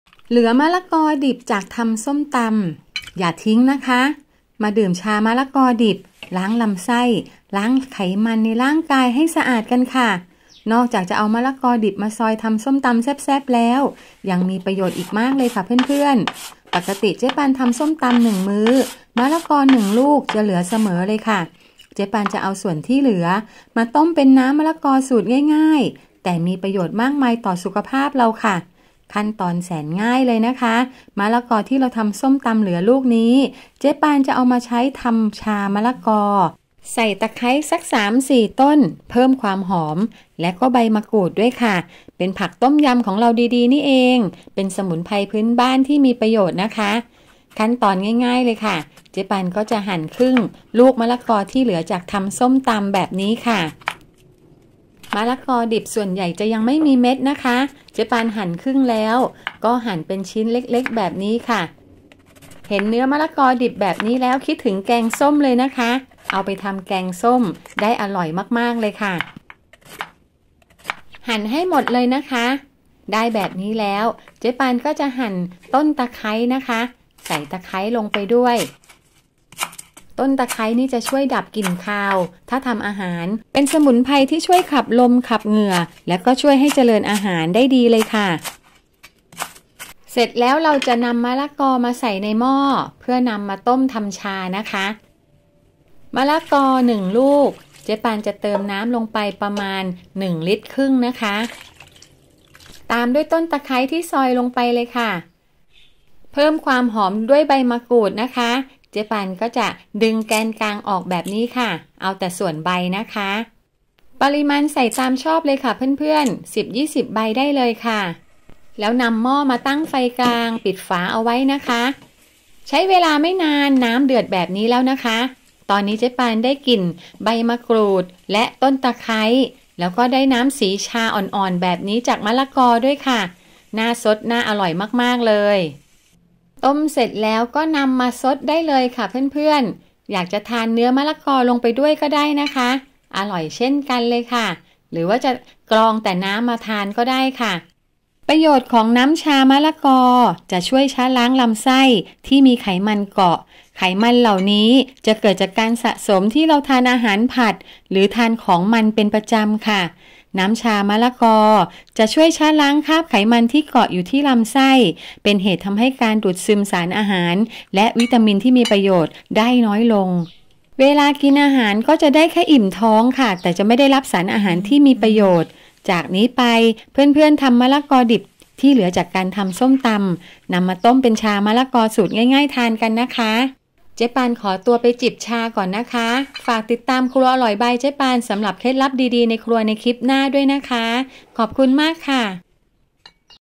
มะละกอดิบเหลือจากทำส้มตำ อย่าทิ้ง!!ทำชามะละกอจิบเป็นสมุนไพร ล้างลำไส้กันดีกว่า
มะละกอดิบเหลือจากทำส้มตำ อย่าทิ้ง!!
ทำชามะละกอจิบเป็นสมุนไพร ล้างลำไส้กันดีกว่า